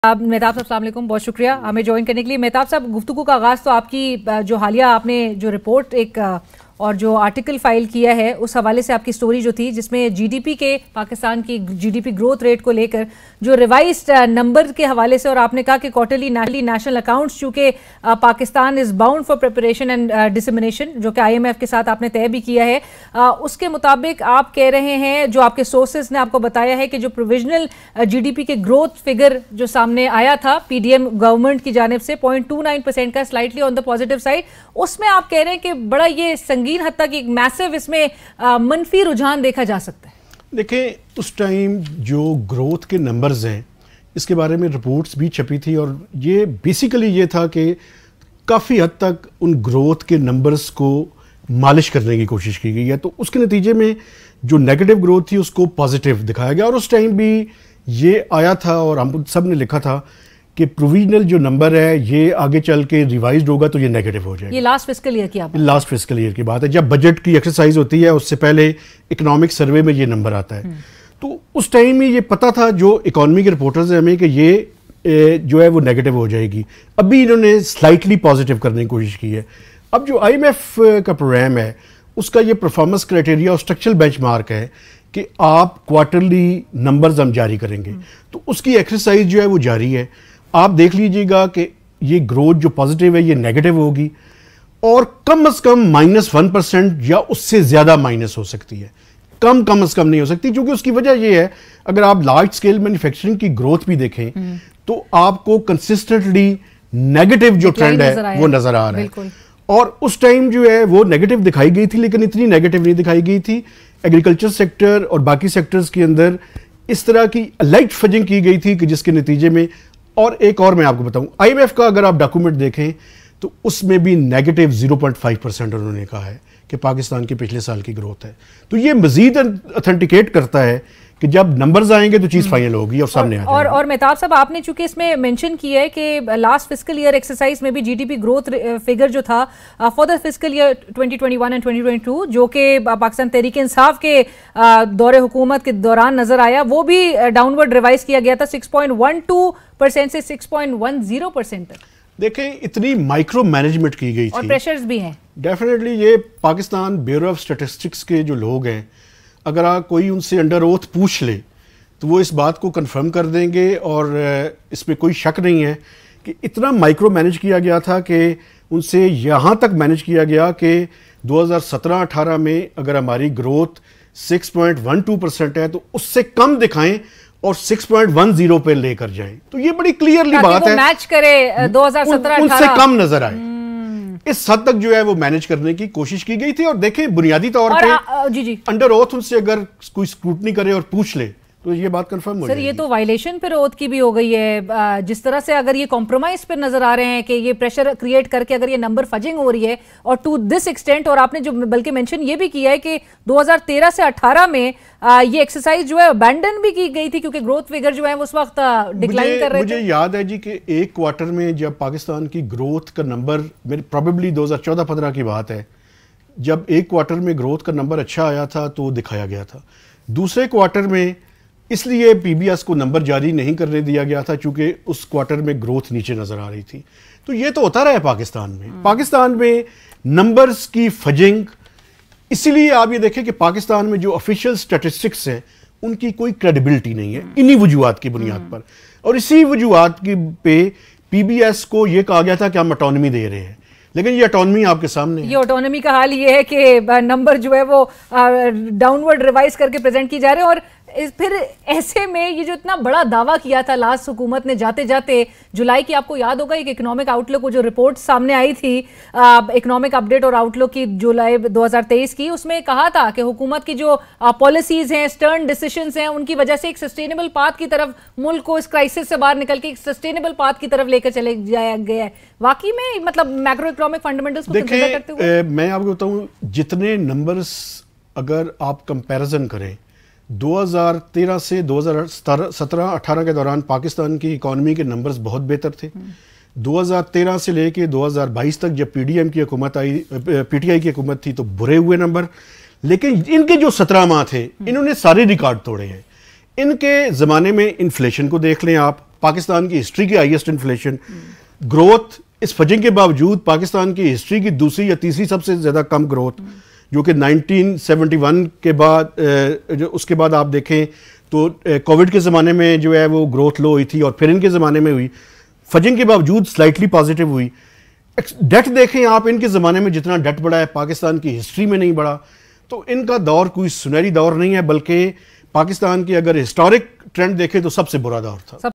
The PDM government's economic indicators are a mess | Mehtab Haider big revelation about PDM
The PDM government's economic indicators are a mess | Mehtab Haider big revelation about PDM || Ausaf Digital